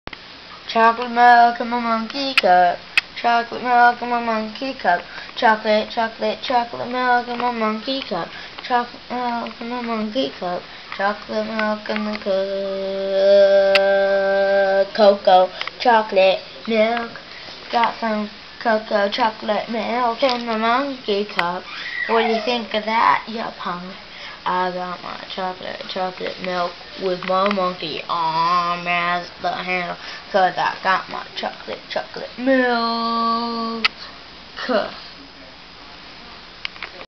Monkey, Arm, and Chocolate Milk
a monkey cup a arm handle ,and a cup of chocolate milk ???!!! a crazy vid